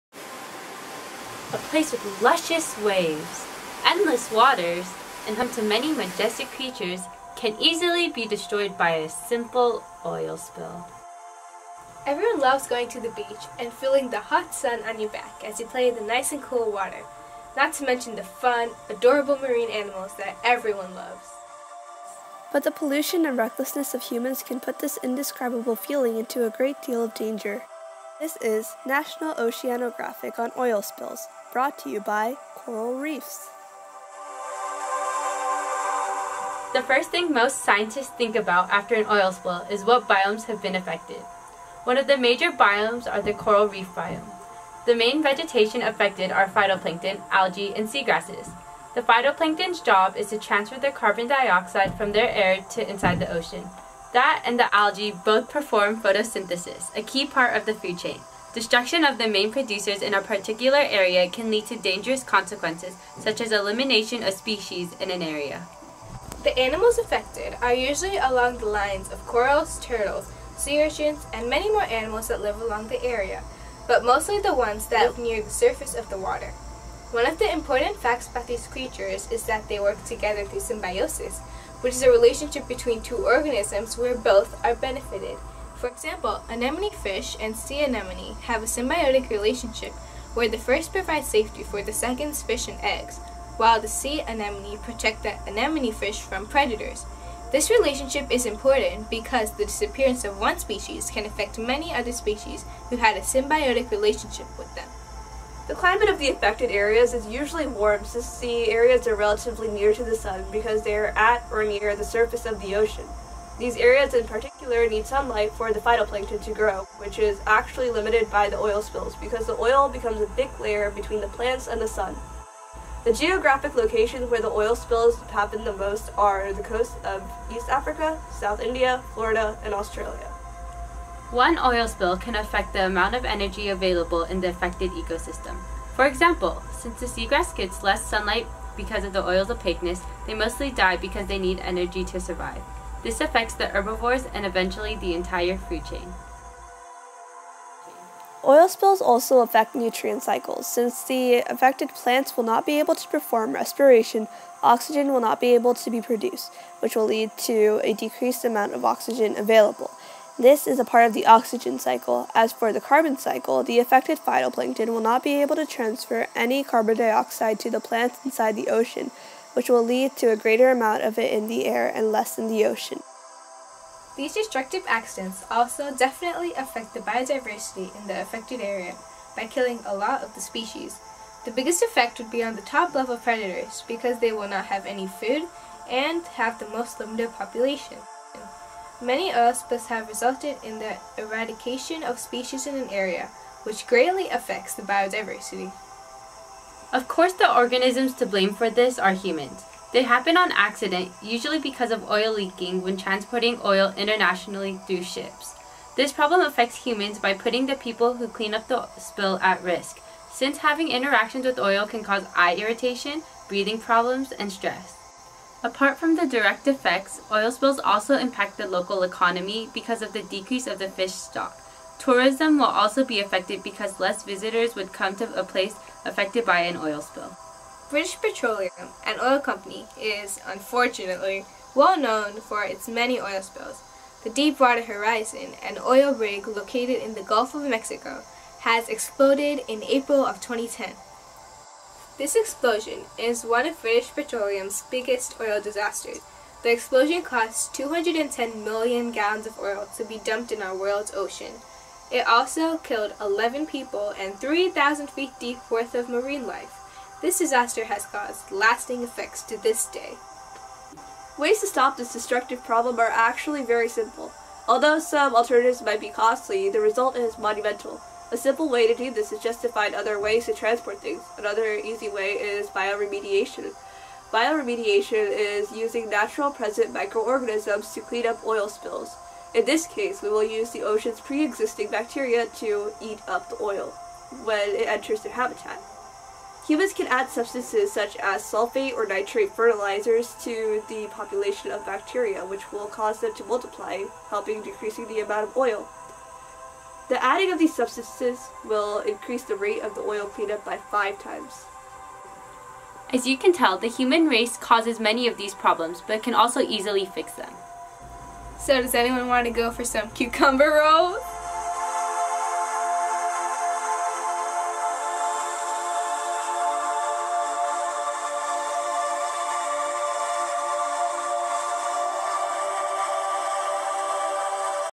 Oil Spills: Effects on Marine Life | National Oceanographic
Torres Biology Mock-umentary (Kimberly, Jainee, and Yusra)